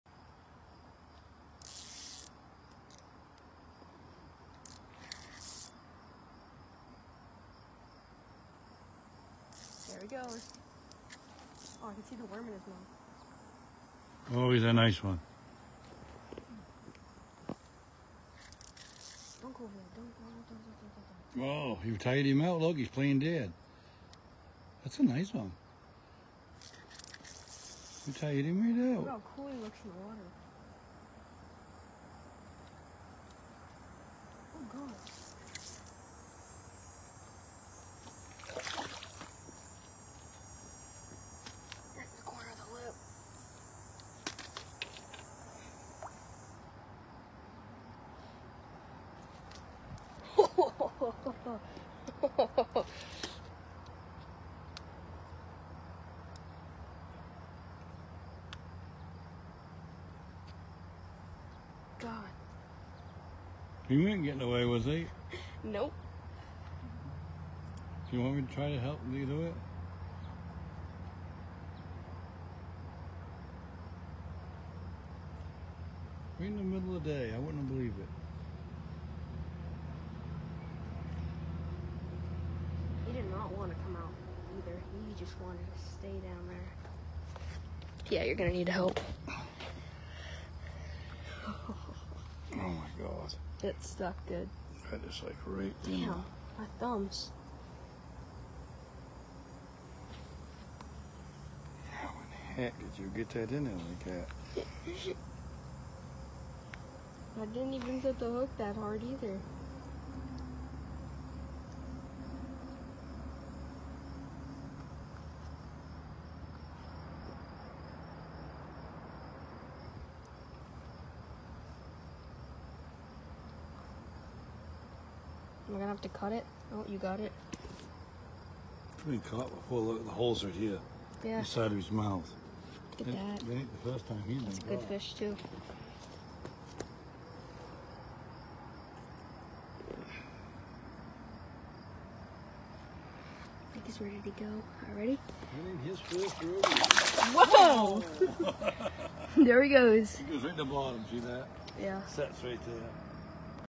Young Angler's Success: Nephew Reels in a Big Bass a Rubber Worm at Adam’s Pond!
This short video shows a special moment between my nephew and his grandpa at Adams Pond. Using a trusty rubber worm lure, my nephew expertly reels in a whopper of a largemouth bass! Grandpa's guidance and a well-chosen bait sure paid off! This is a memory they'll both cherish forever. (Filmed by Gramps)